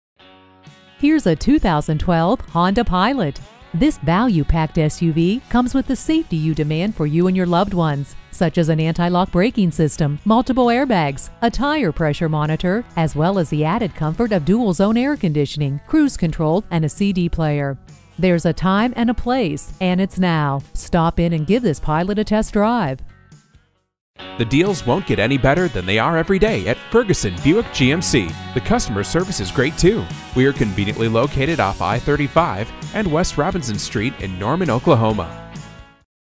Used 2012 HONDA PILOT EX Norman OK Oklahoma-City, OK G2004A - SOLD
Year: 2012
Make: HONDA
Model: PILOT EX
Engine: 3.5 Liter 6 Cylinder
Transmission: Automatic
Color: RED
Interior: Beige
Mileage: 99063
Stock #: G2004A
VIN: 5FNYF3H42CB035719

Address:
1015 North Interstate Drive